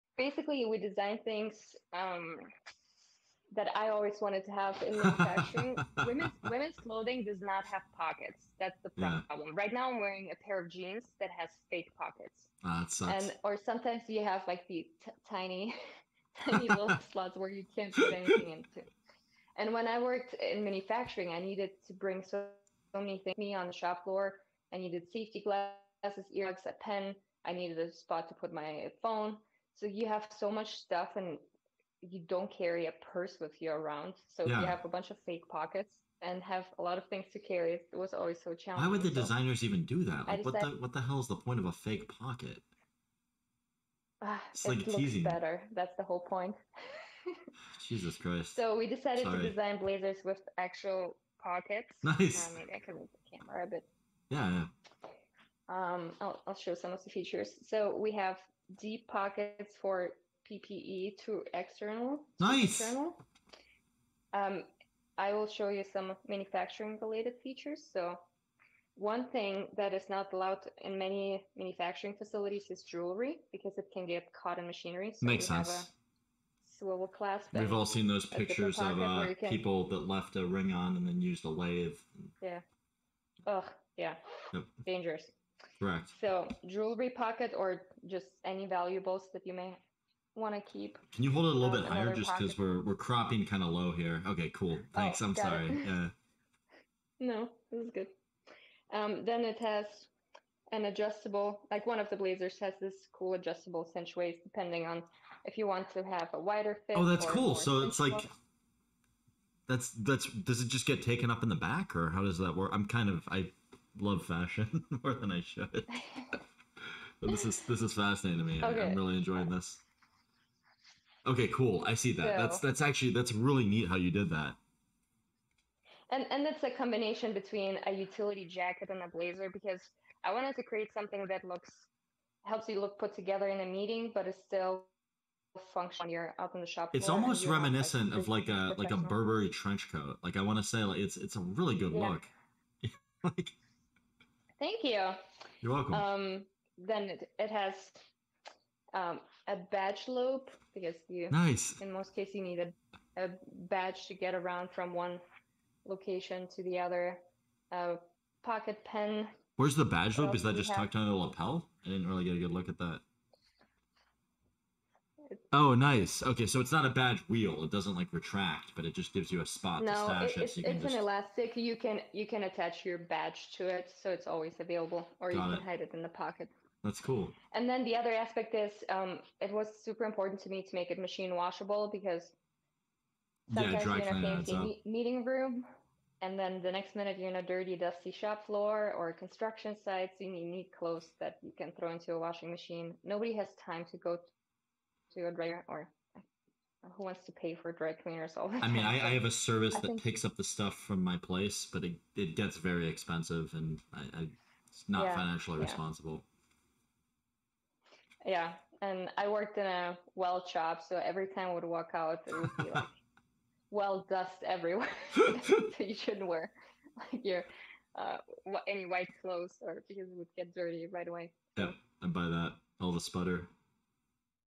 When Life Gives You Fake Pockets, Make Better Clothes
In this just under 4 minute clip, Ana shares her inspiration for creating Xena Workwear. Ana's goal is to use fashionable clothing to make women's workdays run more smoothly.
 Xena Workwear aims to create fashionable functionality, durability, and to include ever-elusive pocket space in their designs. When you have to look nice for a meeting at 8 and be in the shop by 9, Xena Workwear has you covered!